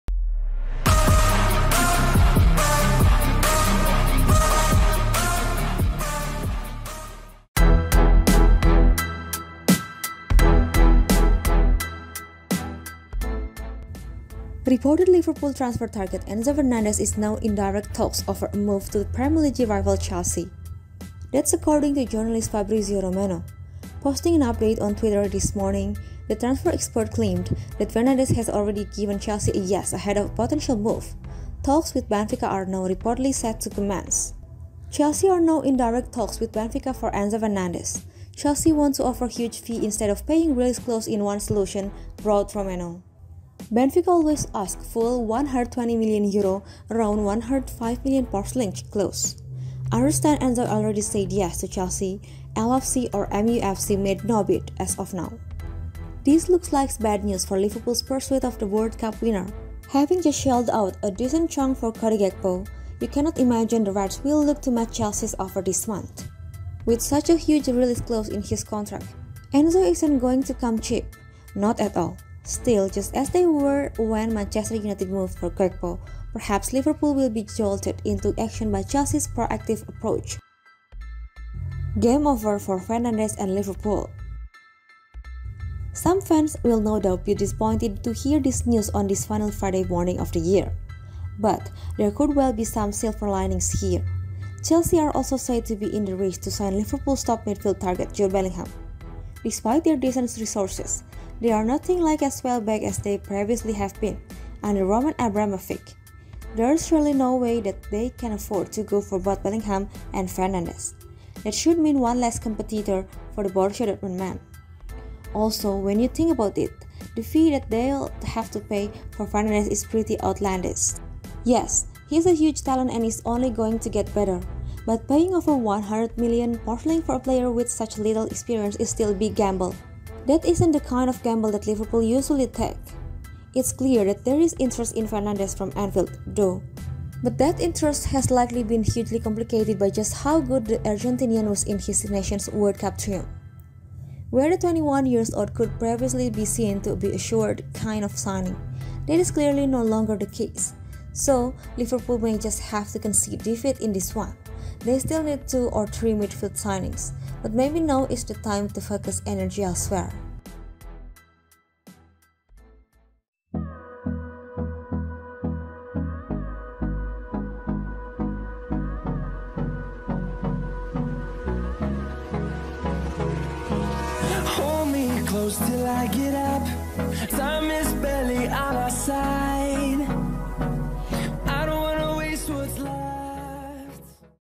'GAME OVER': £105m Liverpool Target Has Just Agreed To Join Chelsea, They're In Direct Talks Now
Reported Liverpool transfer target Enzo Fernandez is now in direct talks over a move to Premier League rivals Chelsea.

That’s according to journalist Fabrizio Romano. Posting an update on Twitter this morning, the transfer expert claimed that Fernandez has already given Chelsea a ‘yes’ ahead of a potential move. Talks with Benfica are now reportedly set to commence.